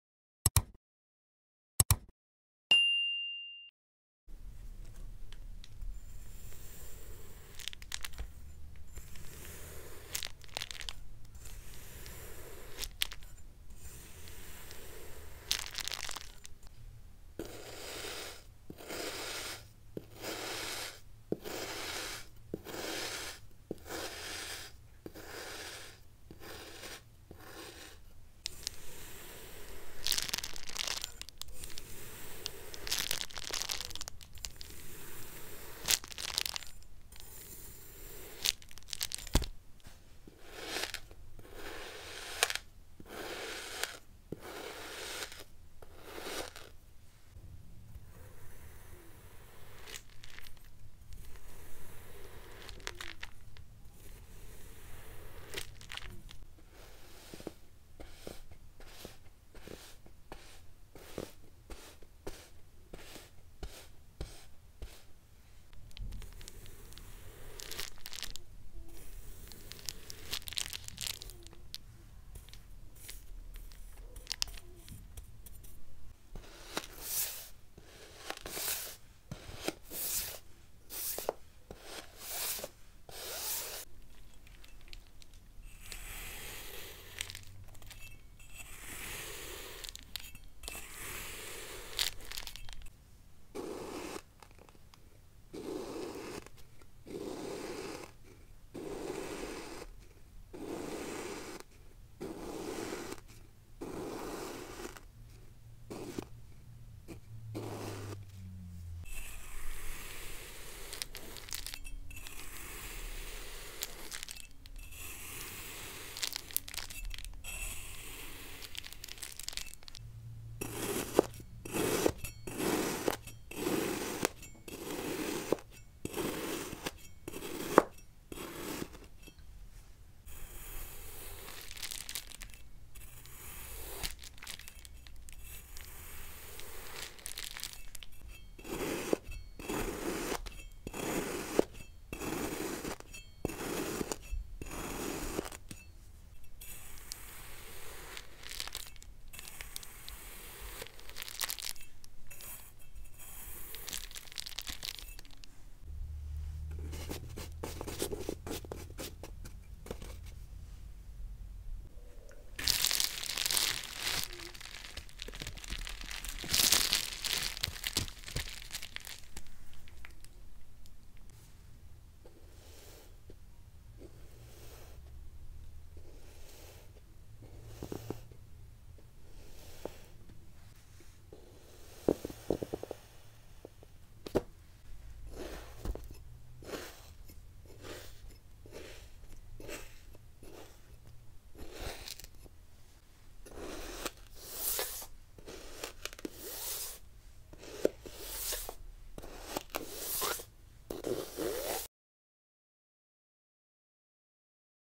Very Satisfying and Crunchy ASMR Kinetic Sand That Makes Relax &Relieve Stress
Cut, slice and dice different shapes of kinetic sand and feel the amazing antistress ASMR satisfaction. Many different types of objects available to cut! With just a knife hit Feel the immense satisfaction!

8119